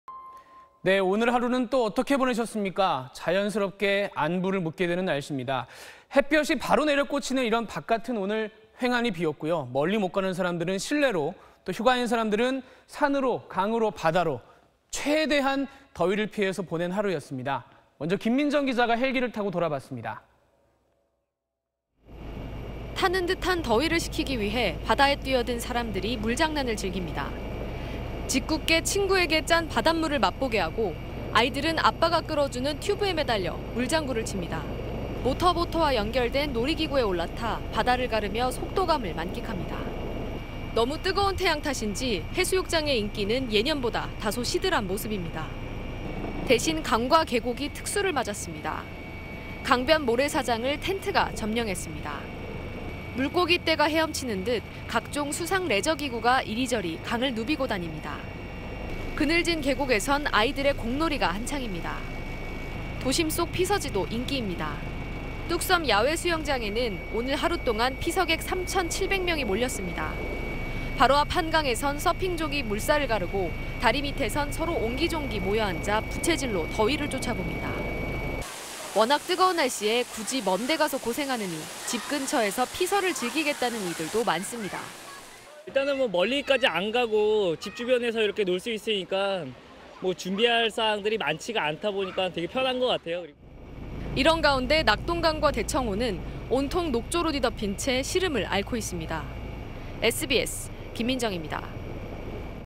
텅 빈 도심…산으로 강으로 수영장으로 '폭염 탈출' / SBS
오늘(29일) 하루는 또 어떻게 보내셨습니까. 자연스럽게 안부를 묻게 되는 날씨입니다. 햇볕이 바로 내려 꽂히는 이런 바깥은 오늘 휑하니 비었고요. 멀리 못 가는 사람들은 실내로, 또 휴가인 사람들은 산으로 강으로 바다로 최대한 더위를 피해서 보낸 하루였습니다.

먼저 김민정 기자가 헬기를 타고 돌아봤습니다.